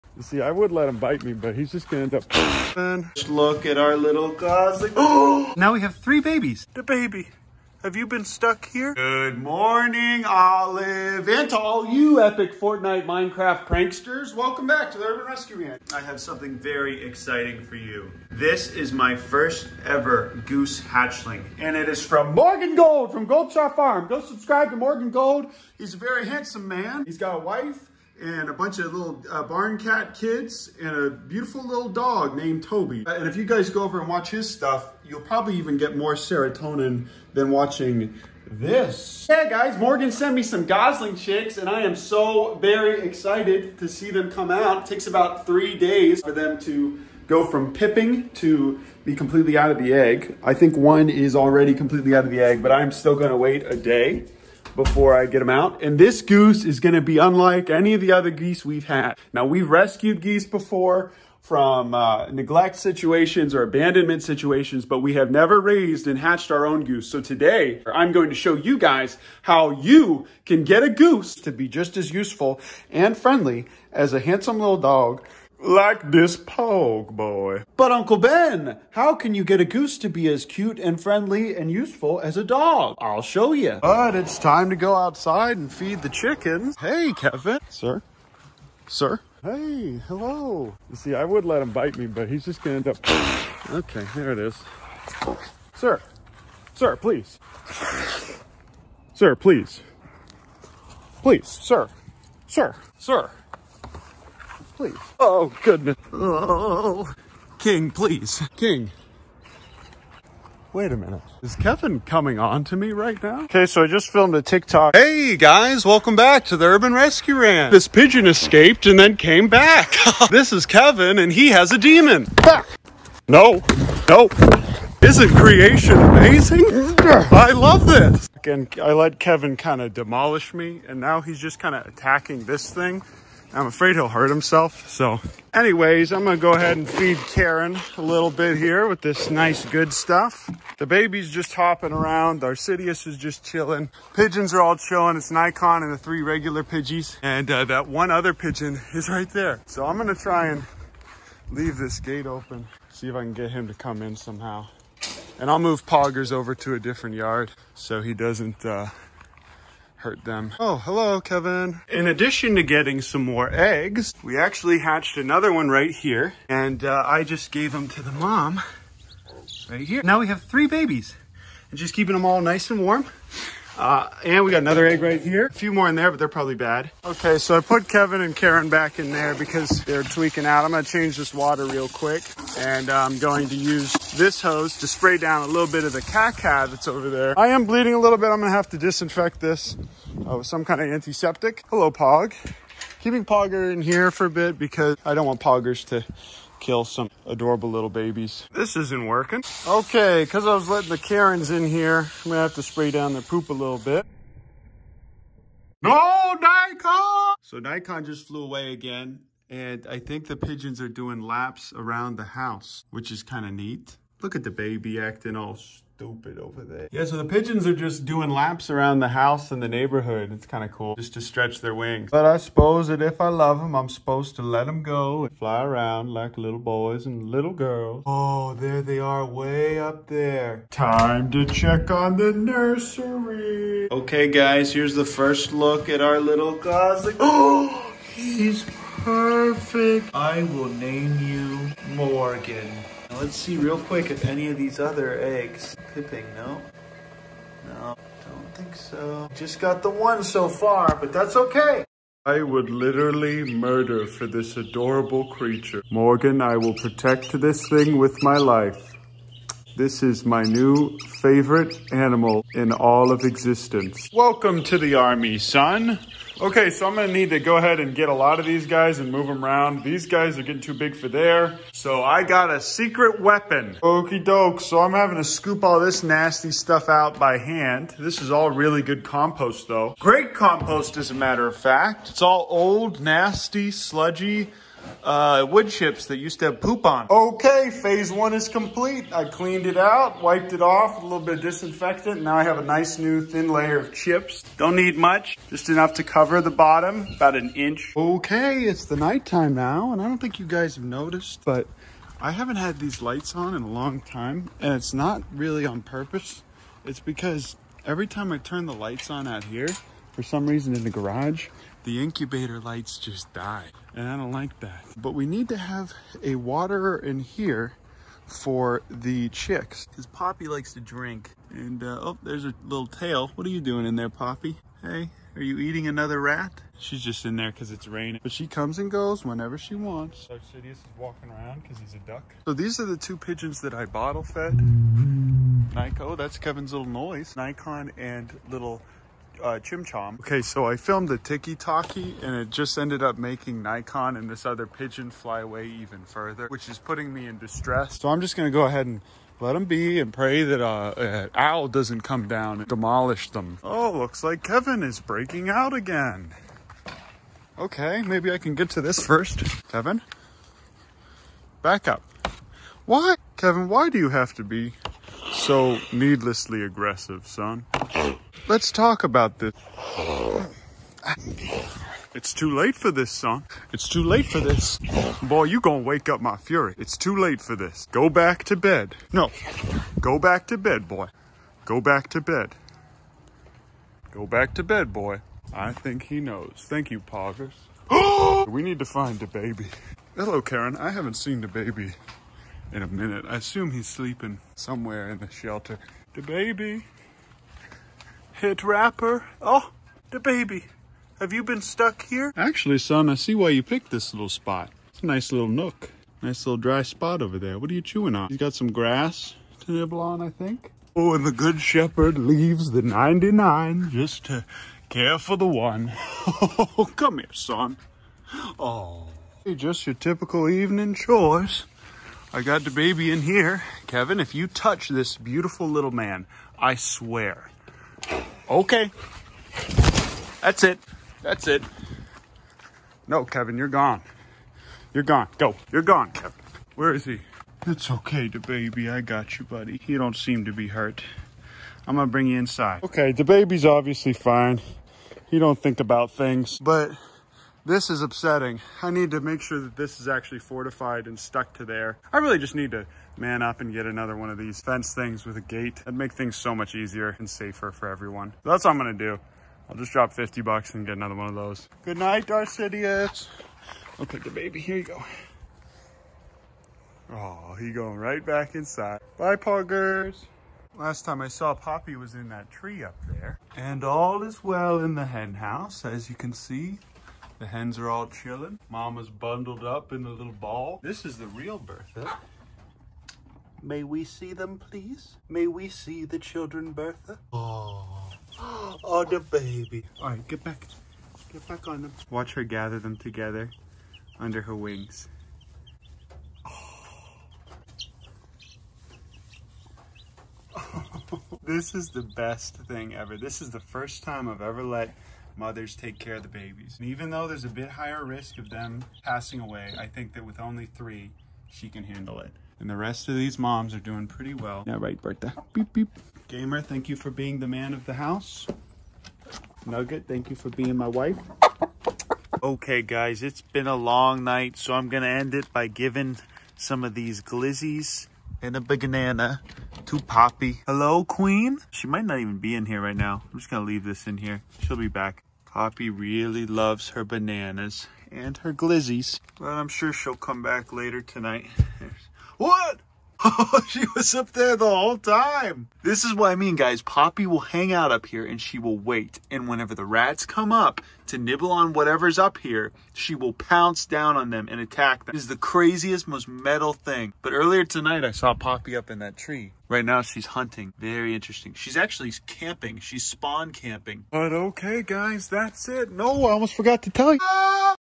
How to Survive a Rhea Attack p2 (+ My First Gosling!!!)
OOOH BOY WE GOT A DOOSIE TODAY SONS. I have a new son, i got attacked by Kevin twice, we got another chick, the pigeons started racing around the house, AND I lost and found Dababy and protected him from a demon!

Love,
Uncle Farmer Dad Ben👨🏻‍🌾🤝❤️

Learn more about us and how to support/volunteer at urbanrescueranch.com !